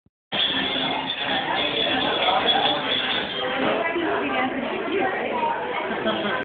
Atheena performing at Mina Dimi's
Atheena solo belly dancing and playing zills at a restaurant in San Antonio, TX USA in 2005.  Become a fan on facebook of "the atheena show" or join us for more info.